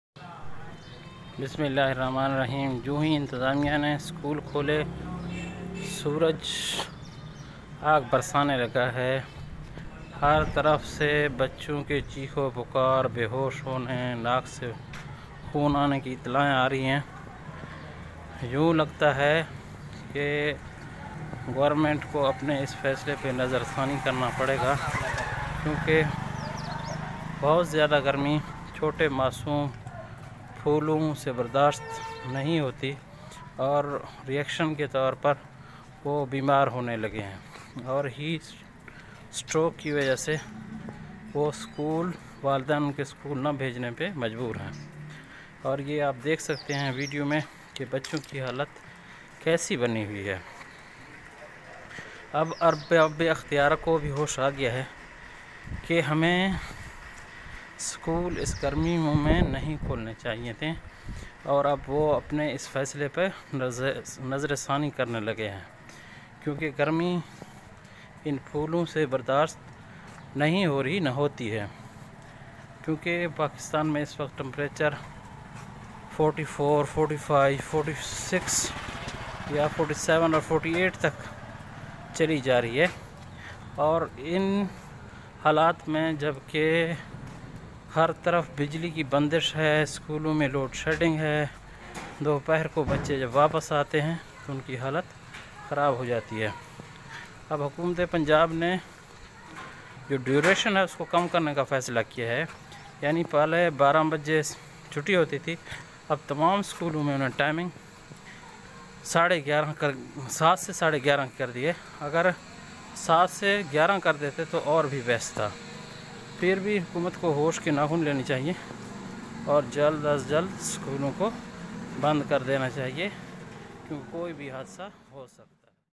the sun is burning every thing | heat stroke | effecting school going students
the summer is on full swing and effecting school going students very much.the matter is very serious.